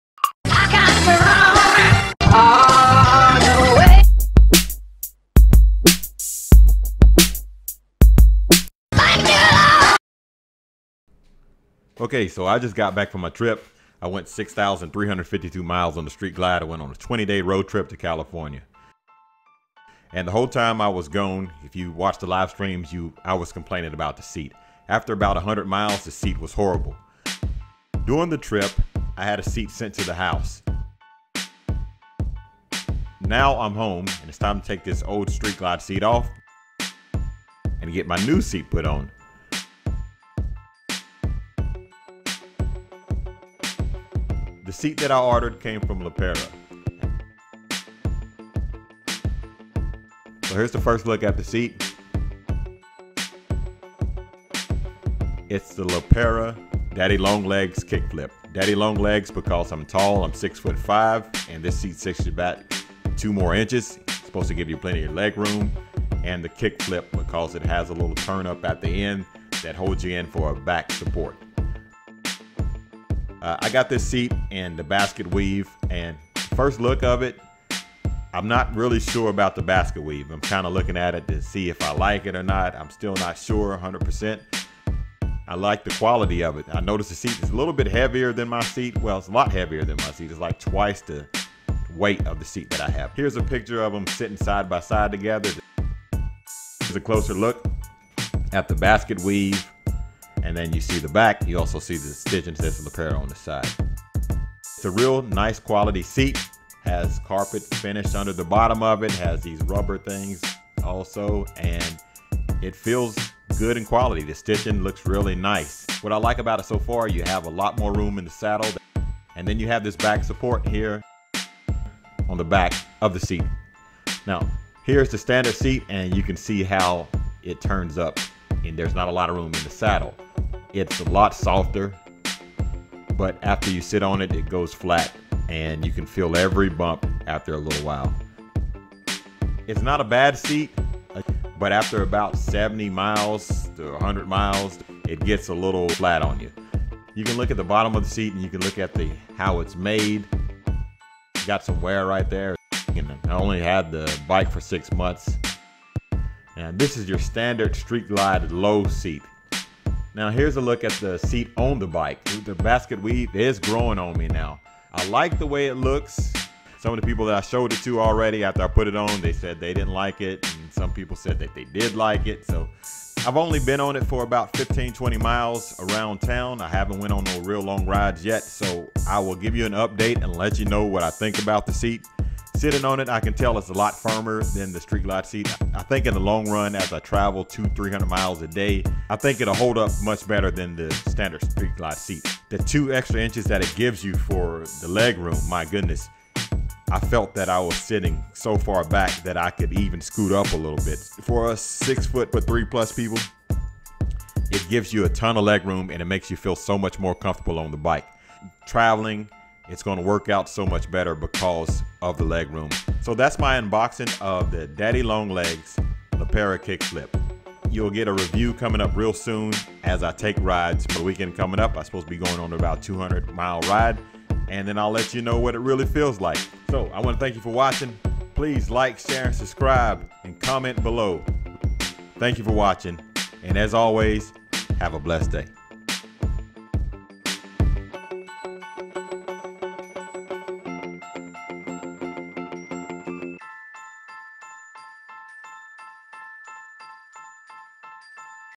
Changing the stock Harley-Davidson seat and upgrading to a New LePera Seat on my 2019 Street Glide.
Changing out my stock Harley-Davidson Street Glide seat for a New LePera Daddy Long Legs Kick Flip made for tall riders.